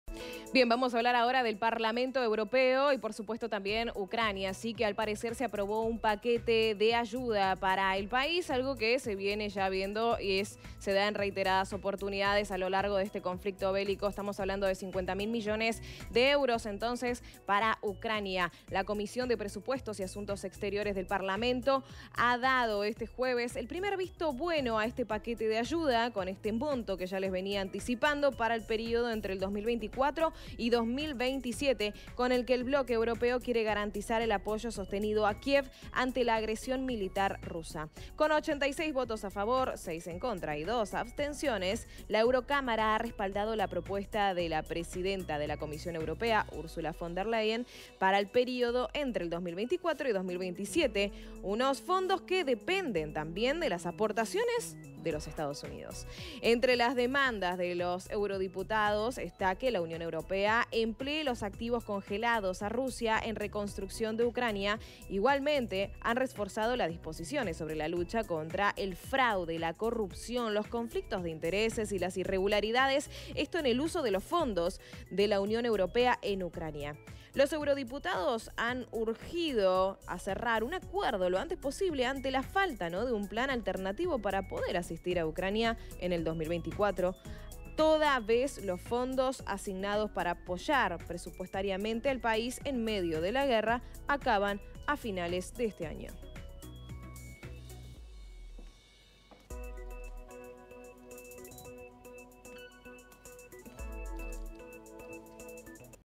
La Eurocámara da el primer visto bueno al paquete de 50.000 millones de euros a Ucrania hasta 2027
👉 La comisión de Presupuestos y Asuntos Exteriores del Parlamento Europeo ha dado el primer visto bueno al paquete de ayuda a Ucrania de 50.000 millones de euros para el periodo entre 2024 y 2027.